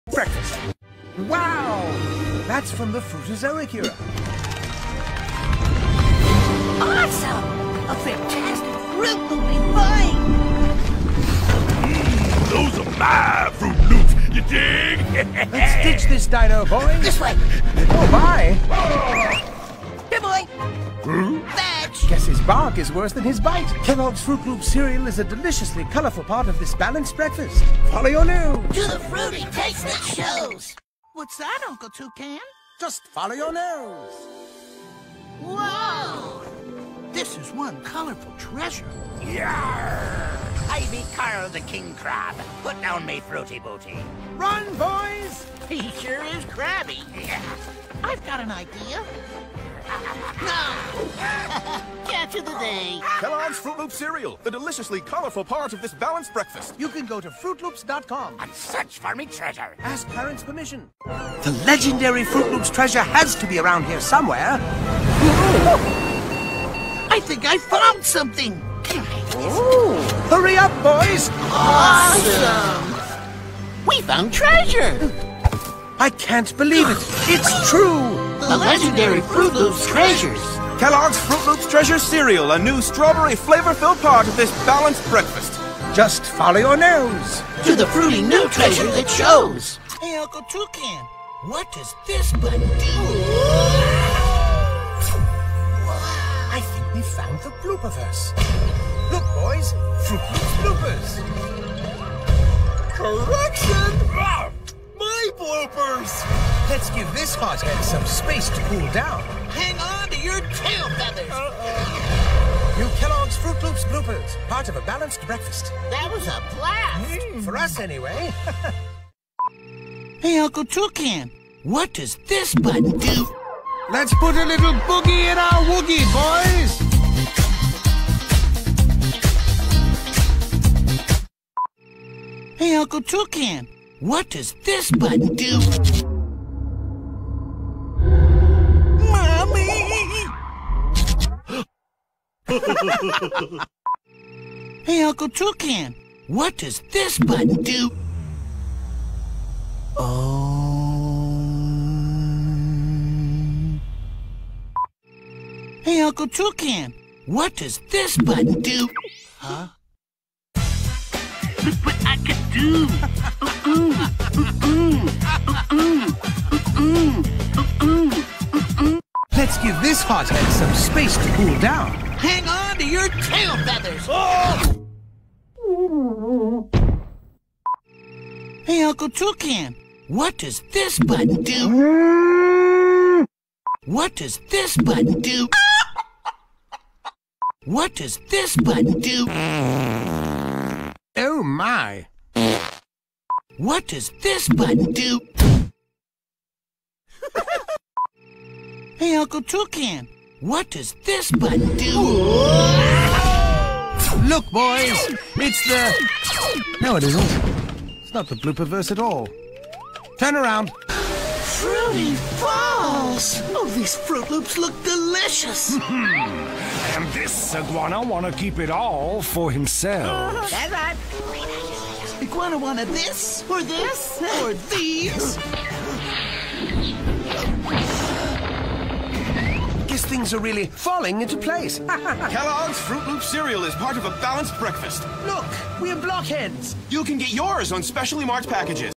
😎TOUCAN SAM AND THE BOYS "IN LOST TREASURES"😎
😉VENTURE WITH TOUCAN SAM AND THE BOYS WHILE FINDING TREASURE, PIRATES, INTERSTELLAR SPACE, AND OF COURSE
 OH MY!!  WHO FAAAARTED  ?